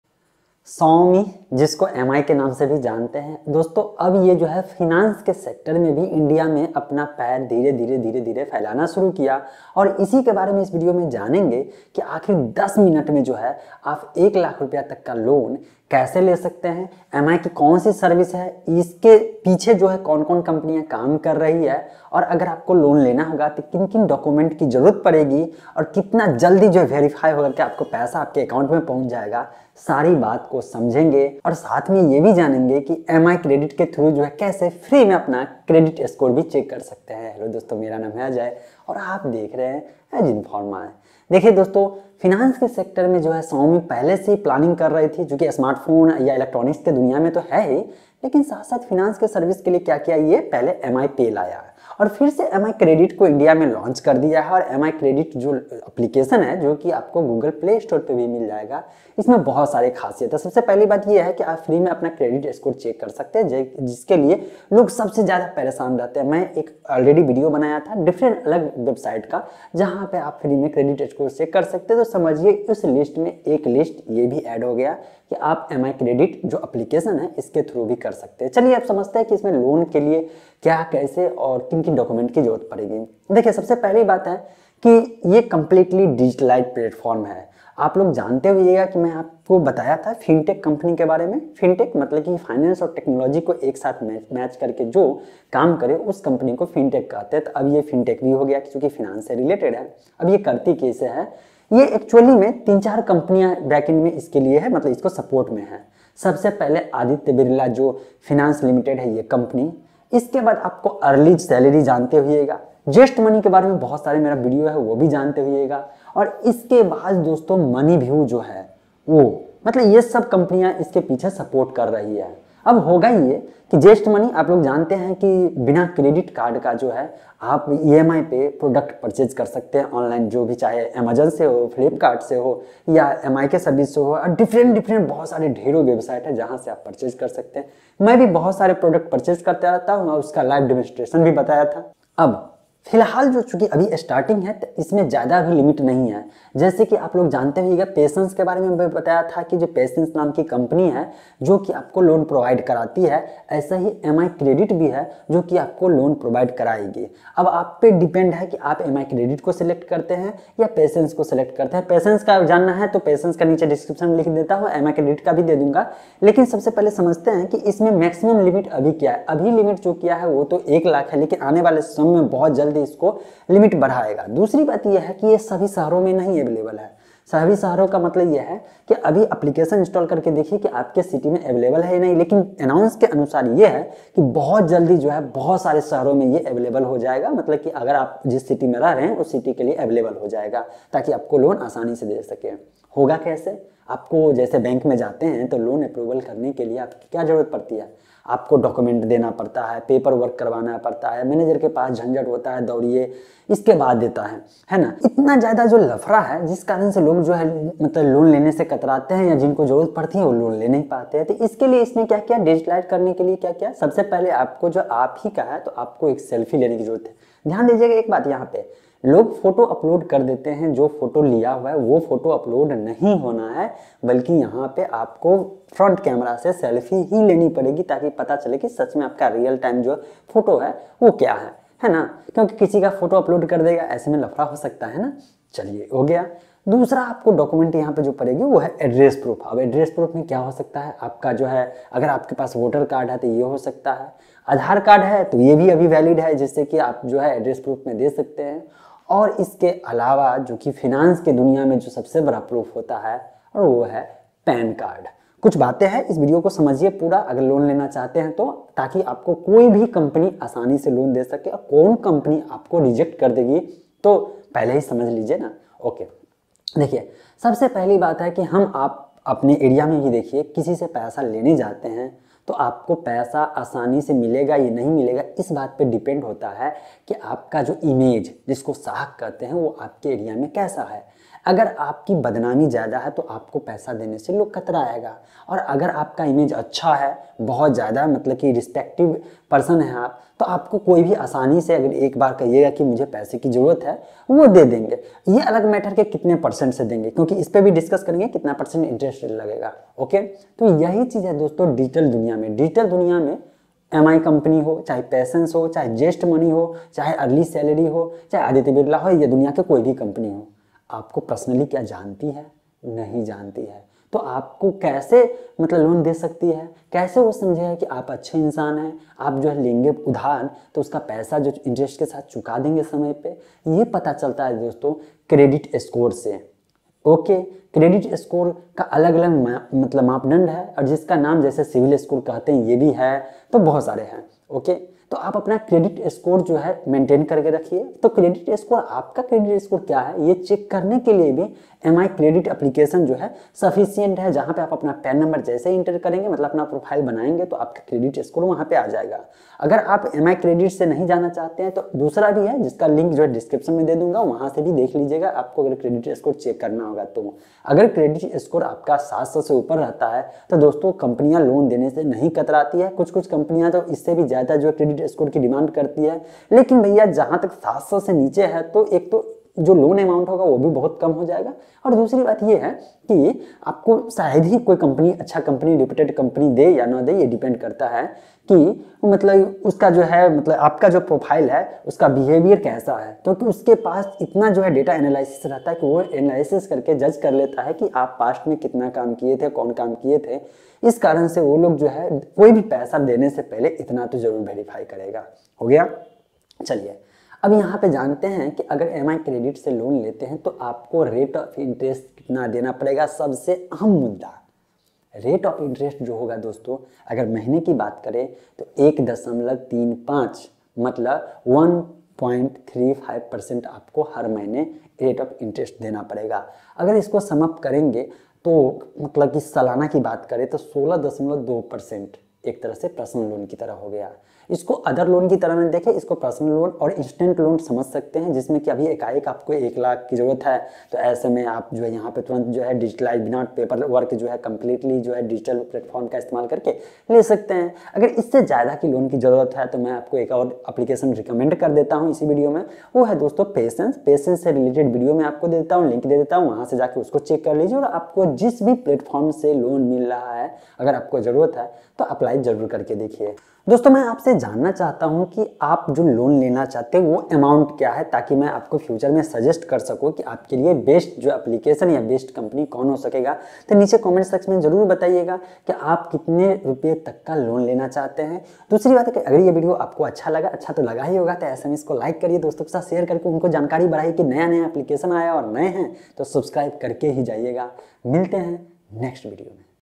10 मिनट में 1 लाख रुपया लोन ले वो भी बिना किसी पेपर वर्क के | Loan Kaise Le बिना झंझट के Mi Credit
Agar aap bhi loan lena chahte hai to Xiaomi ne MI Credit app launch kiya hai jisse aap upto 1 lakh rupees loan le sakte hai |

Aap Apna credit score yaha check kar sakte hai

Dosto, kya aap bhi bank ke manager se paresha hai ki wo loan jaldi approve nhi karta hai aise me bahut sare fintech company aaya hai jo ki aapko instant personal laon provide karati hai usi me ek name aur add hua hai jiska name Mi Credit hai.
What about your opinions? tell me in comment.
About : AS Informer is Technology and Business channel , you can watch tech  information videos in Hindi .